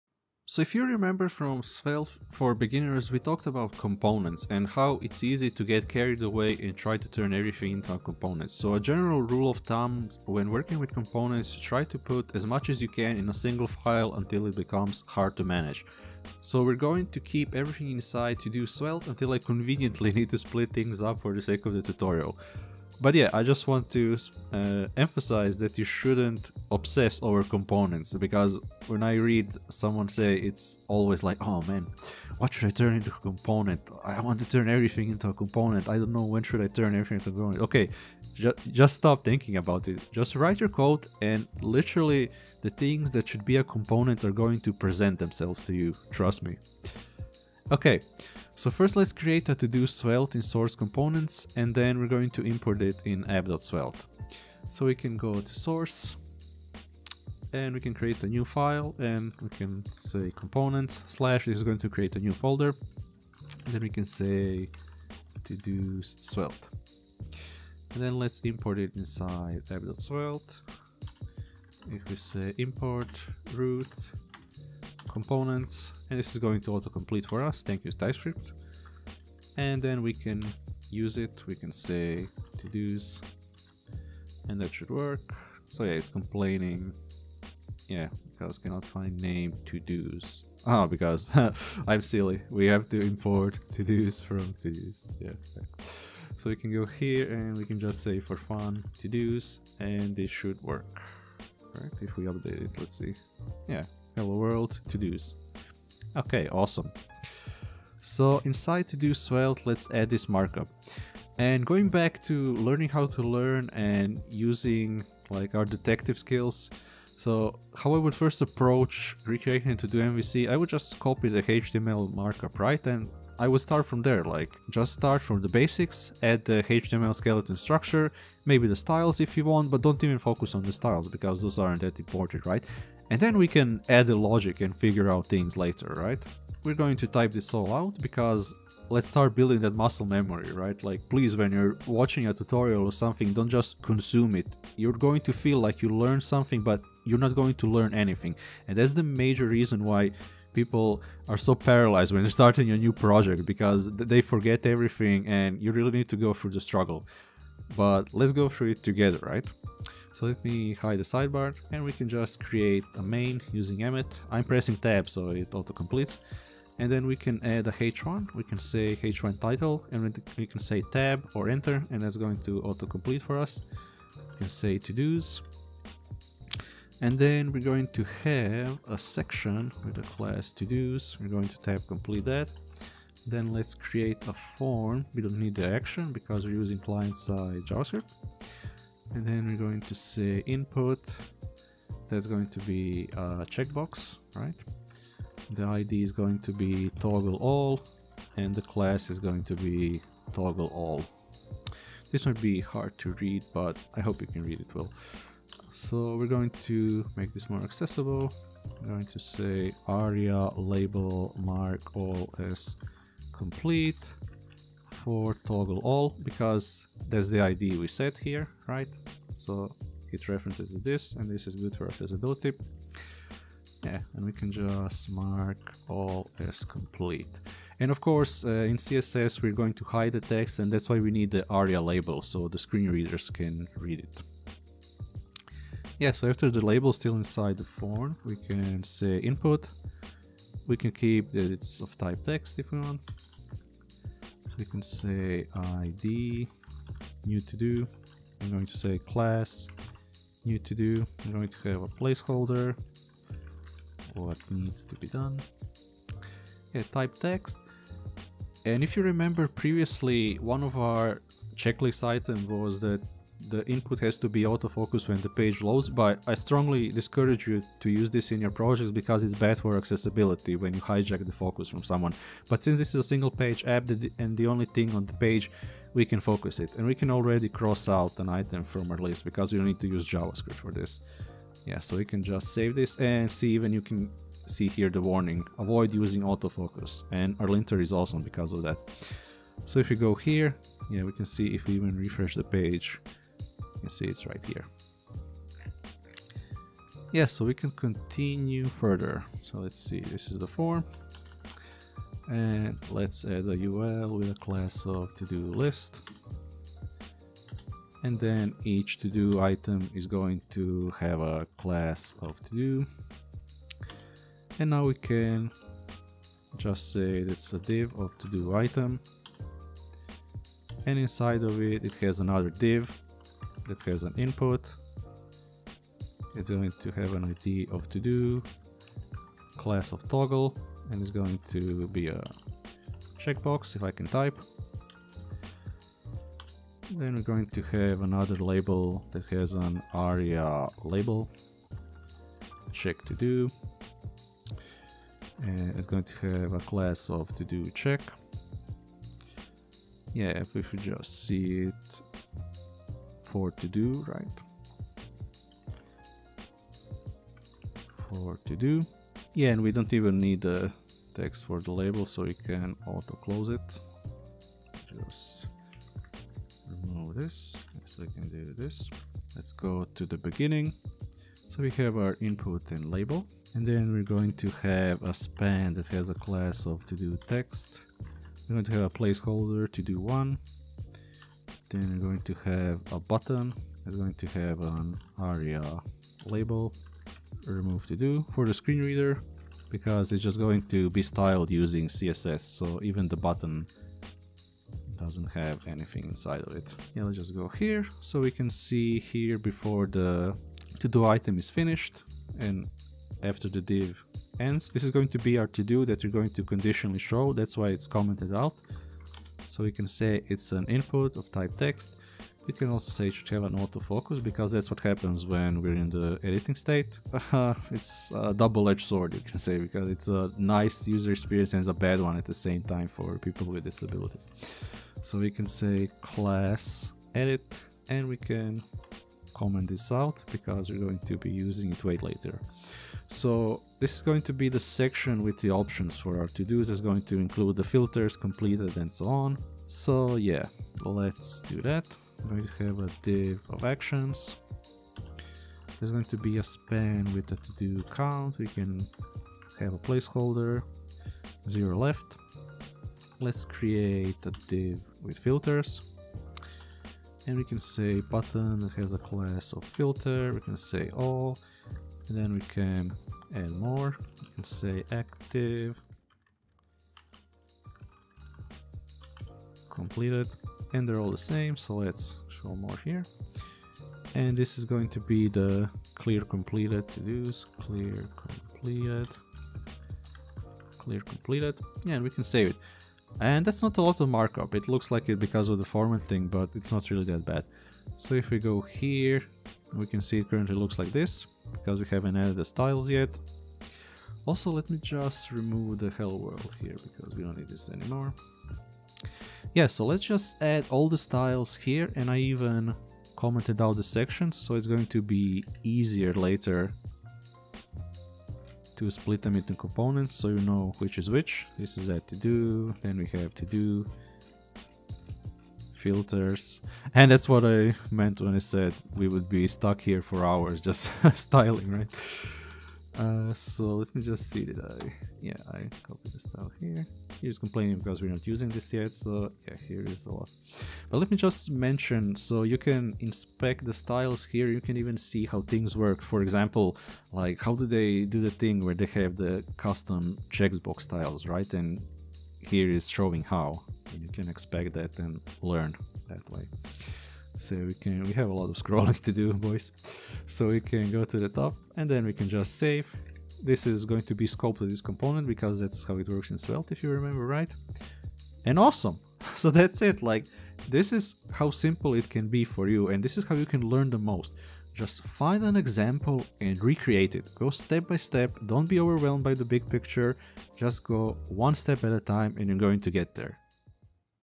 Make a Svelte Todo App #6 - Markup
In this video we're going to add the HTML markup of our Svelte todo app.